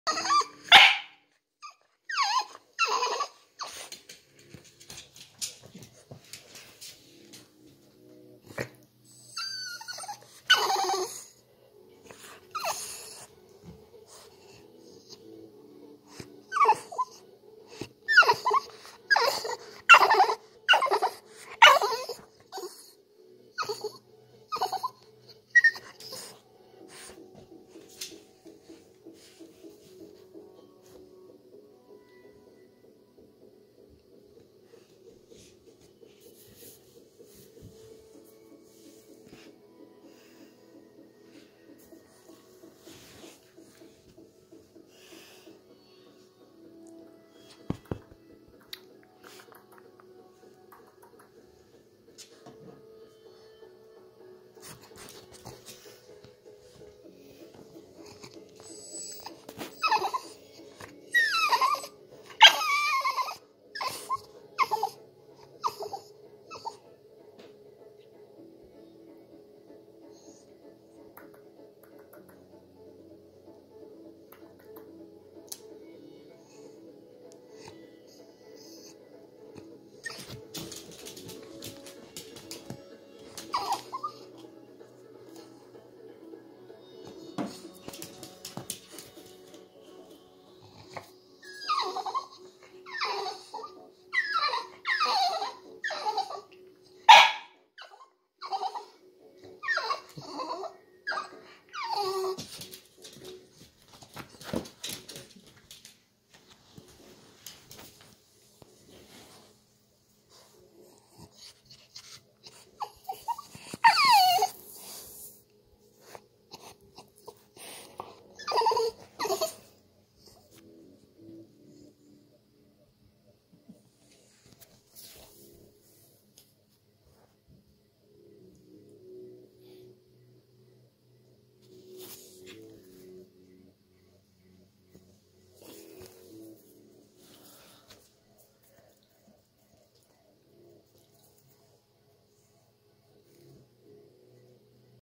french bulldog wants to get on the couch
this funny french bulldog is moka and he wants to get onto the couch